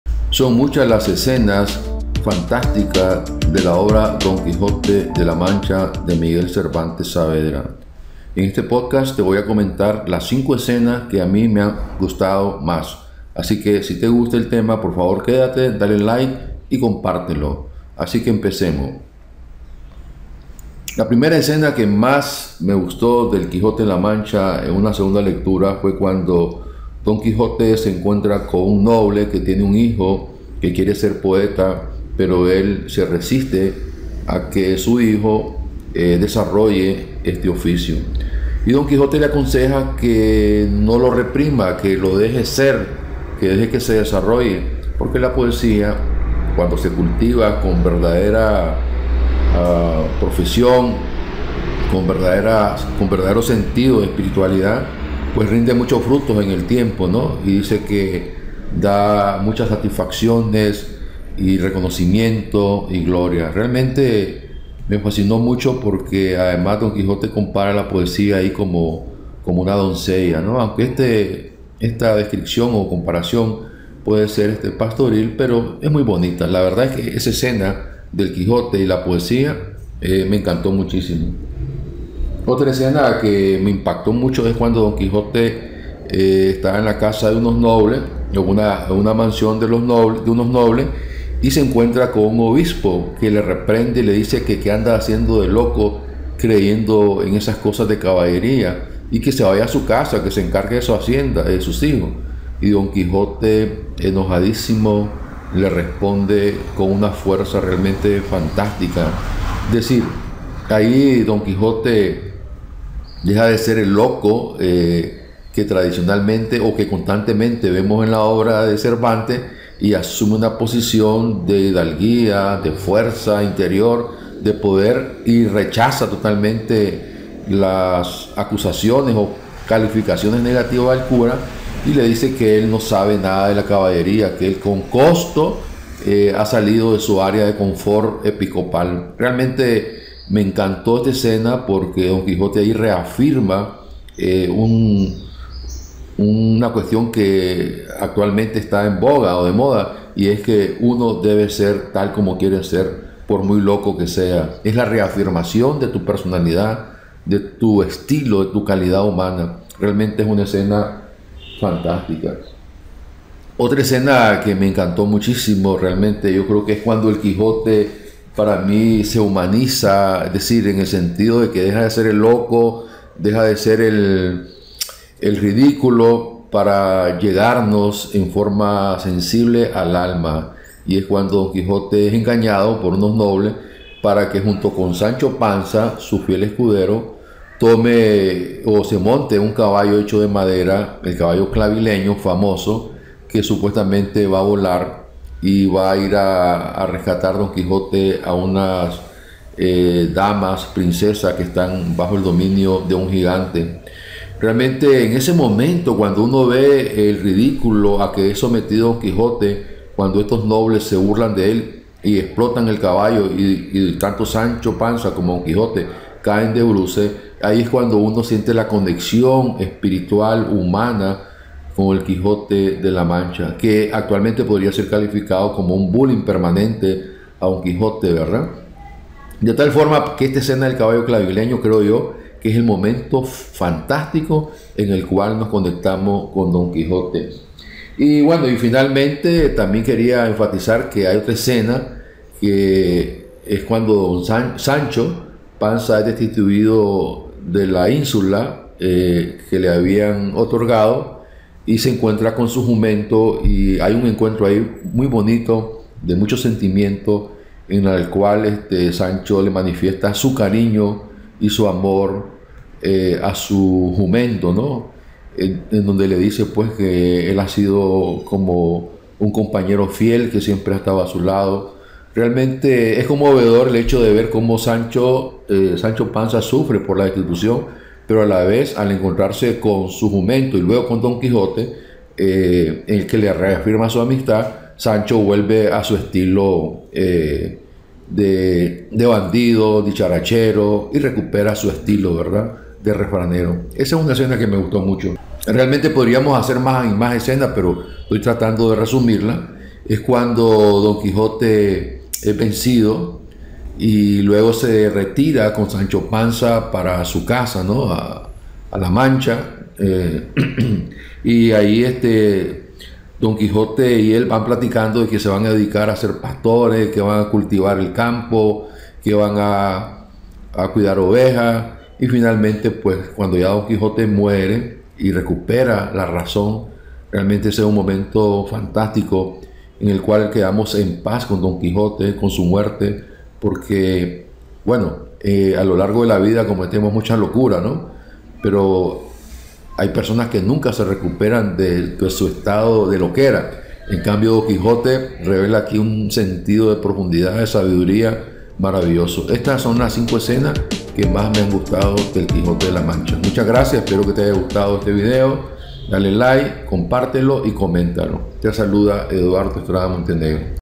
Las cinco escenas que más me han gustado de Don Quijote
Las cinco escenas que más me han gustado de Don Quijote ha

Las reflexiones sobre la poesía  --a la compara con una doncella--, la confrontación con un obispo católico, la escena del caballo Clavileño, el reencuentro de Sancho Panza con su jumento, y las reflexiones antes de morir, son las cinco escenas que mas me han gustado de Don Quijote de la Mancha.

Acerca del autor del canal:
Blog del  autor: